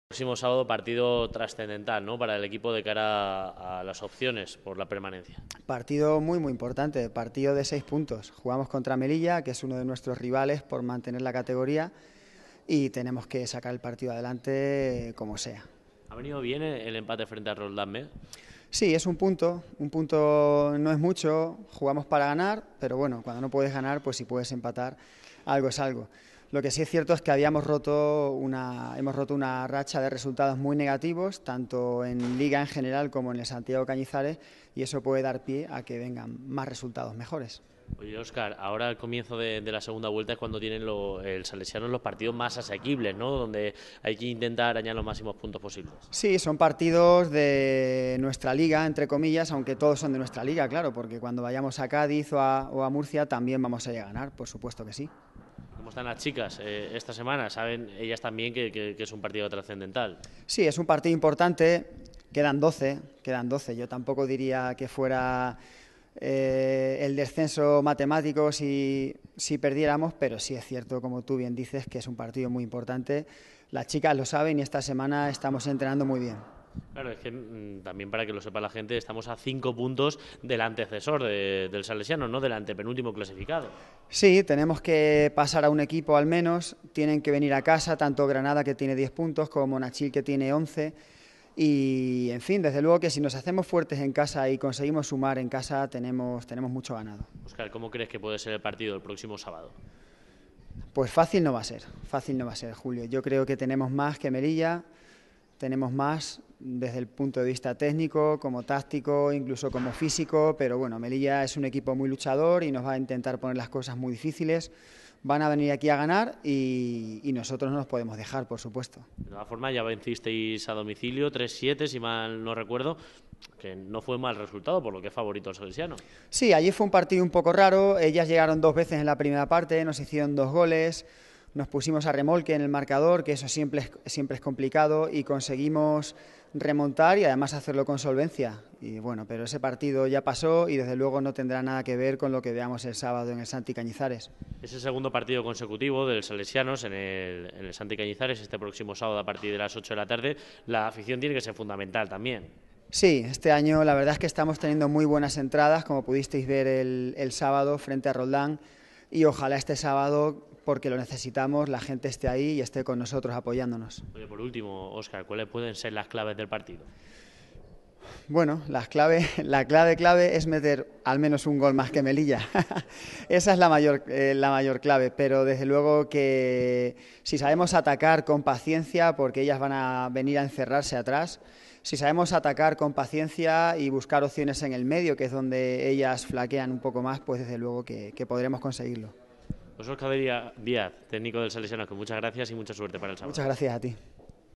DECLARACIONES OSCAR DIAZ
Salesianos Fútbol Sala Femenino se la juega este Sábado ante Melilla. Penúltimo y último clasificado intentarán recortar distancias a sus antecesores. El empate no sirve a ninguno de los dos